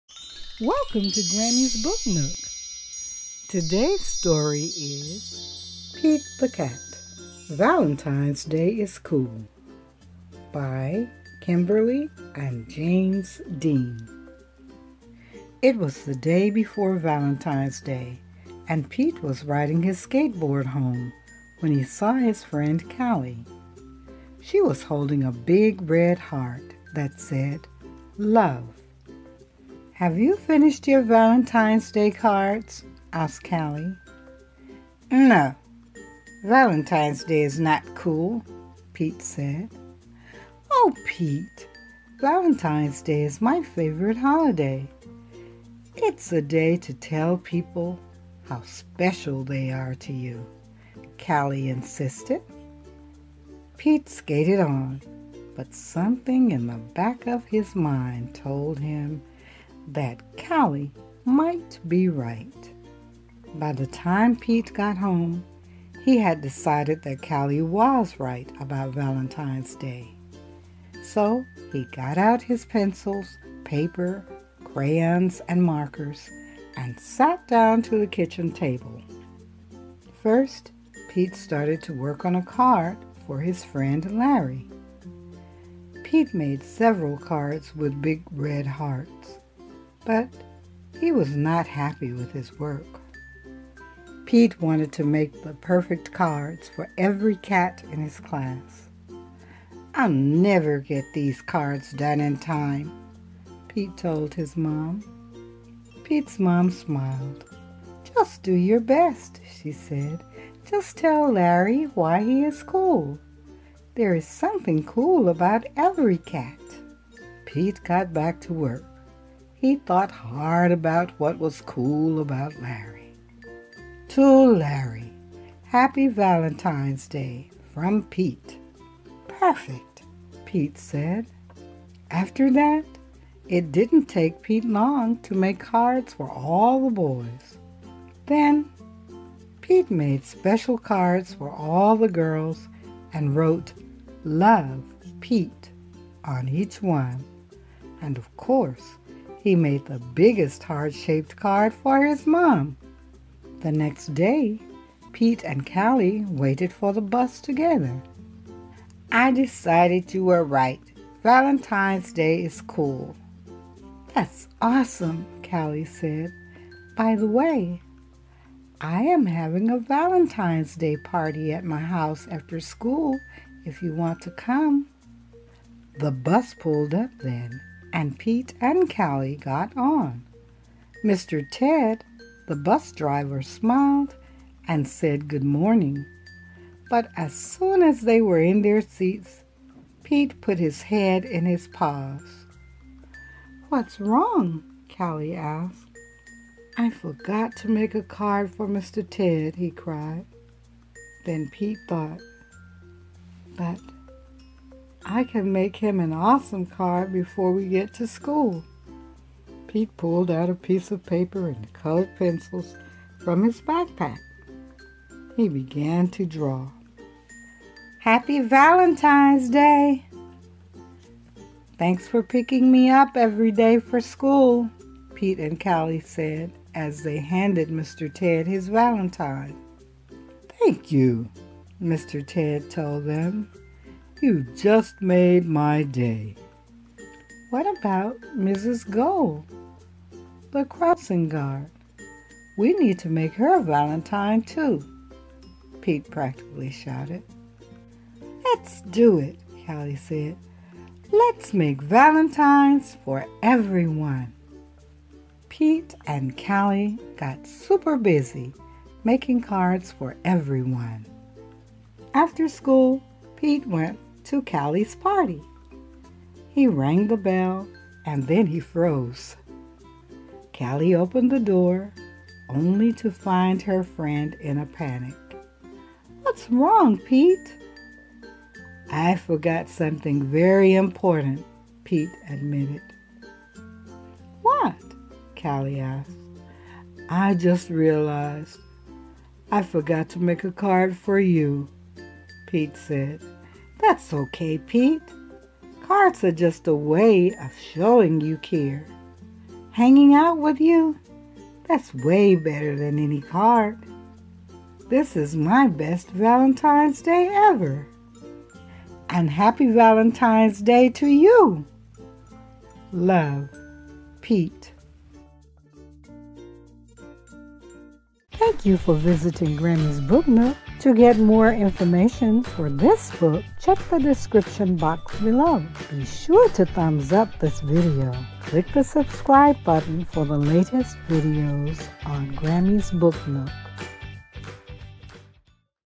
Pete the Cat: Valentine's Day is Cool | Children's Books Read Aloud
Pete the Cat: Valentine's Day Is Cool
by James Dean (Author, Illustrator), Kimberly Dean (Author)

Pete the Cat thinks Valentine's Day isn't cool . . . until he realizes how many special cats there are in his life! Pete works hard to make valentines for everyone, and it turns out to be the grooviest Valentine's Day ever. But what happens when he realizes he's forgotten to make a card for a very important cat? Join Pete the Cat as he discovers just how special Valentine's Day can be!

To purchase this book

Accelerated Reader:
ATOS Book Level: 2.8
Interest Level: Lower Grades (LG K-3)
AR Points: 0.5

Lexile Measure: AD650L